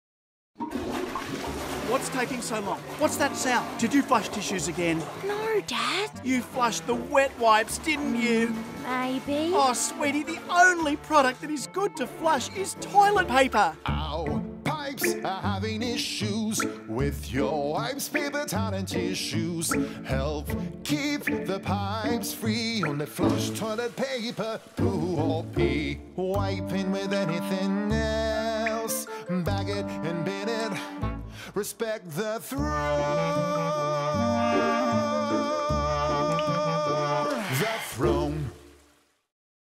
Hunter Water | Respect The Throne 45 second social
30 and 15 second TVC series created alongside this 45 second animation. Used to prevent pipes getting clogged in the Hunter Water network.